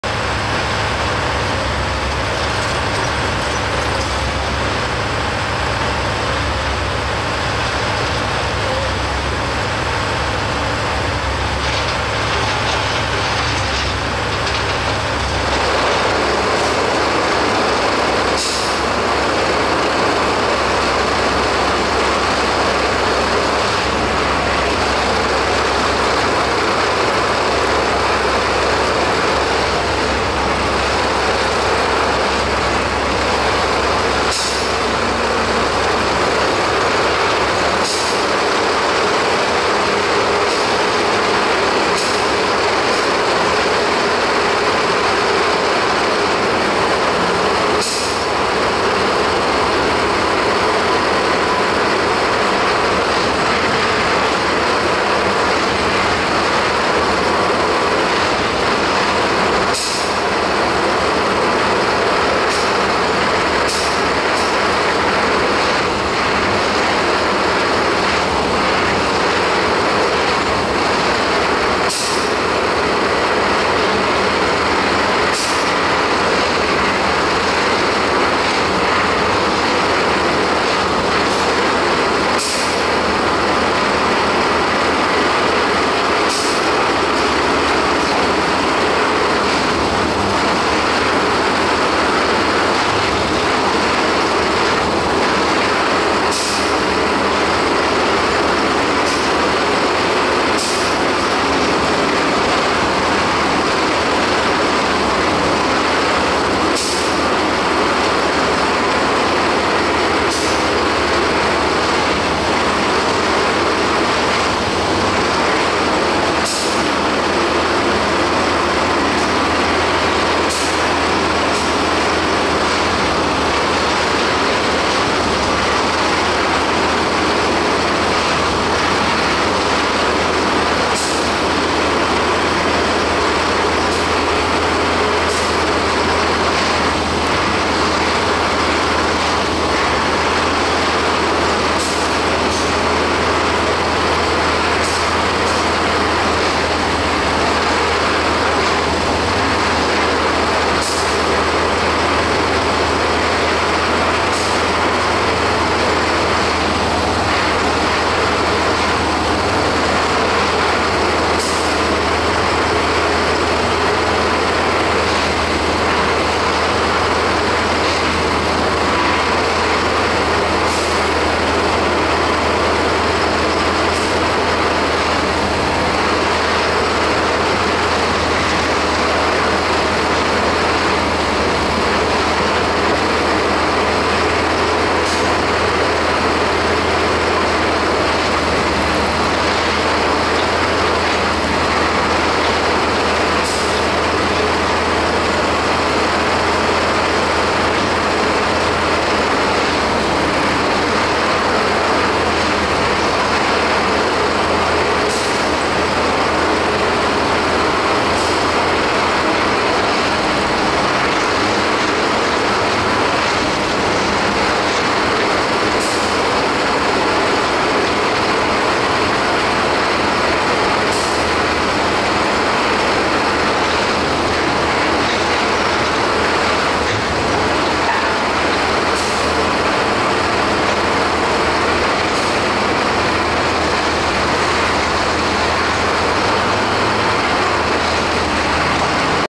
台鐵 瑞穗車站南端 新自強隧道啟用 線路切換工程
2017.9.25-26 台鐵 瑞穗車站南端 新自強隧道啟用 線路切換工程